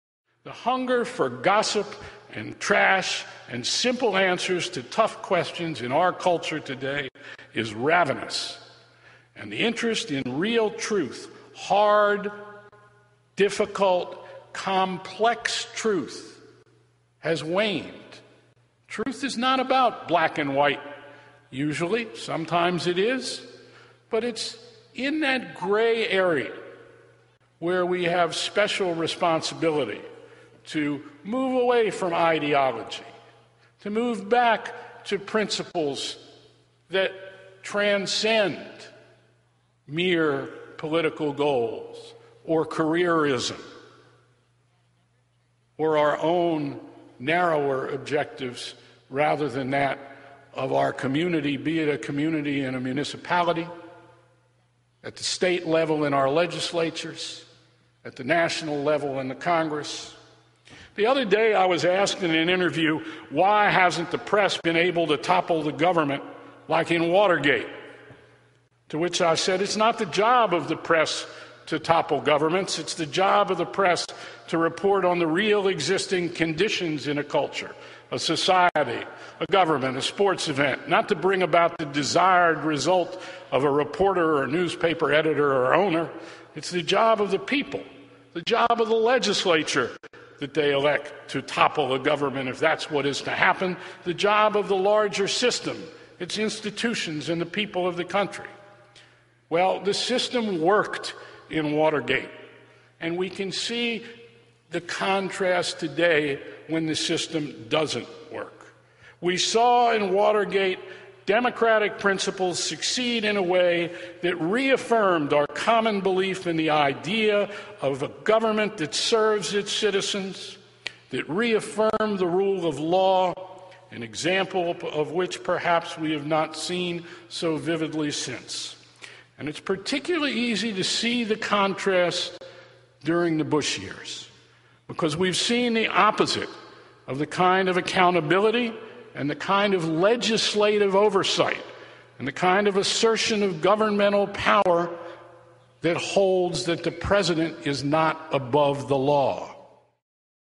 Award Winning Journalist Carl Bernstein
Over the course of his 30-year career, Carl Bernstein has won virtually every prize in journalism, including (with Bob Woodward) the Pulitzer awarded to The Washington Post for its Watergate coverage. Always a principled voice of our time, he once again challenges current events with his new book, A Woman in Charge: The Life of Hillary Rodham Clinton. Much anticipated, it promises to be his biggest blockbuster since his award-winning bestseller All the Presidents Men on the Watergate scandal.